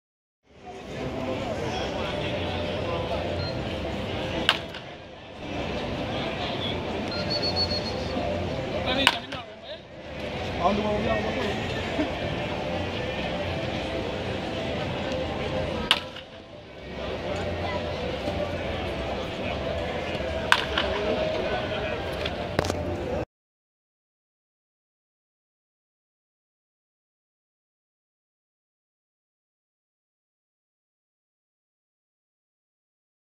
Yorman Rodriguez batting practice at the Futures Game
Cincinnati Reds outfield prospect Yorman Rodriguez taking batting practice at the 2015 Futures Game.